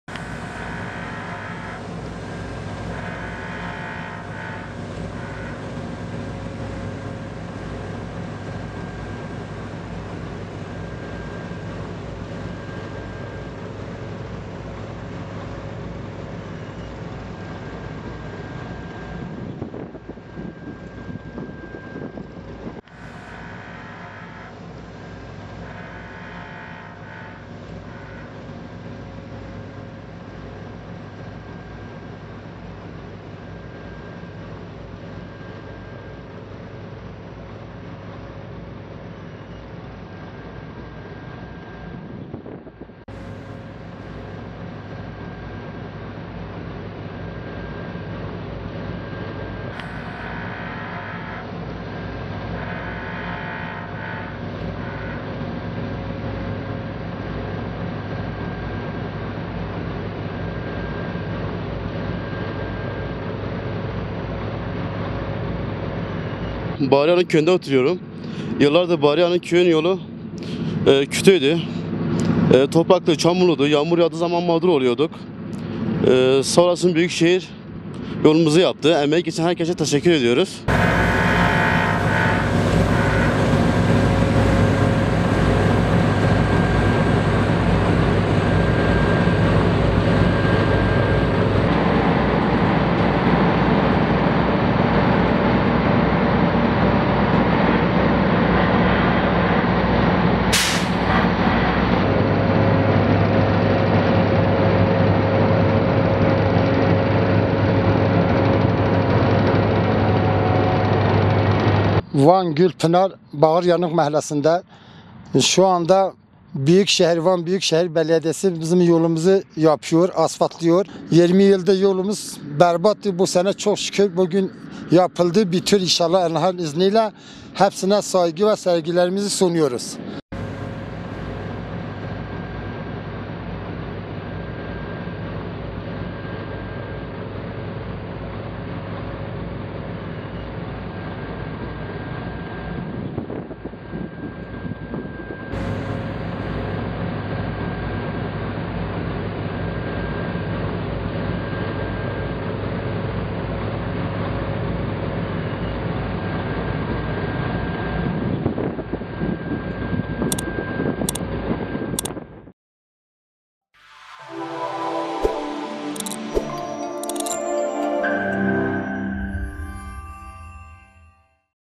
Van'da yeni yol yapım çalışmaları devam ediyor
Van Büyükşehir Belediyesi, Gürpınar’ın Bağrıyanık Mahallesi’nin 20 yıldır süren yol sorununu çözdü.

Değerli İzleyicilerimiz;